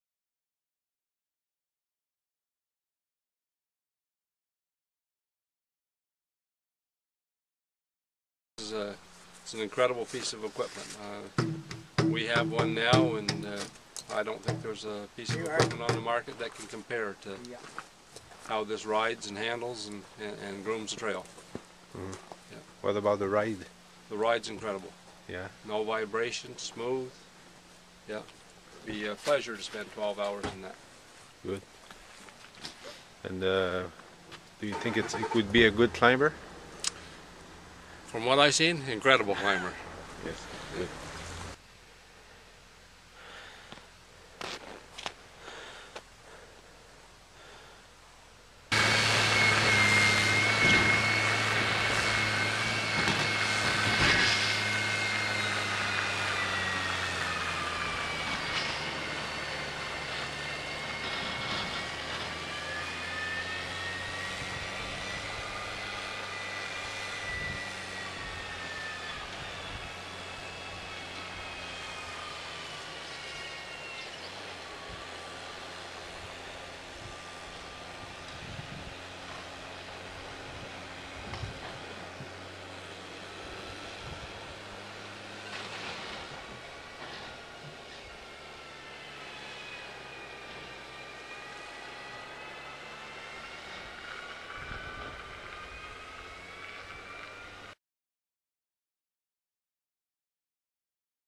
Keswick River Snowmobile Club's testimonial about Gilbert RTS groomer
Carl Fox talks about the Gilbert RTS snowgroomer after a DEMO in the Keswick River Snowmobile Club trail system.